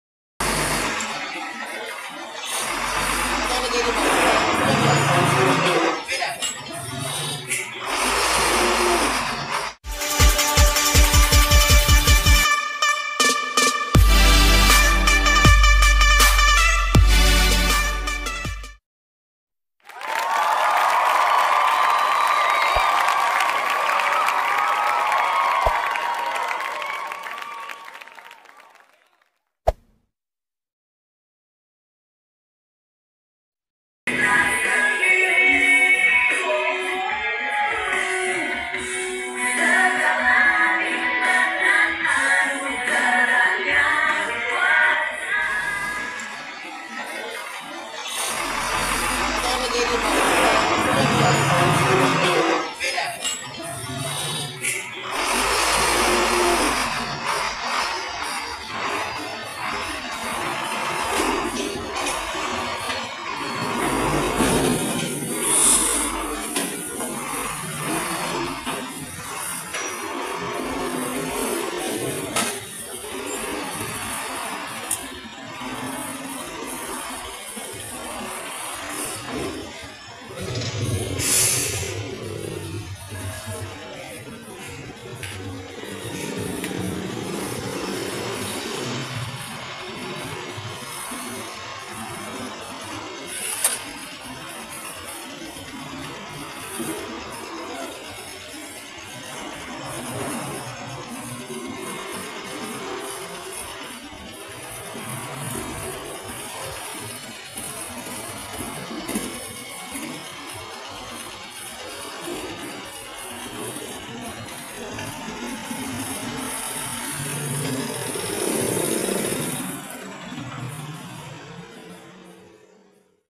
truk besar bawa muatan
truk trailer muatan berat yang sedang mengangkut muatan di pelabuhan kapal Ferry tanjung Serdang batulicin Kalimantan Selatan Indonesia,truk trailer Hino ini berjalan mundur saat mau keluar dari pelabuhan dan sempat truk trailer oleng karena jalannya sempit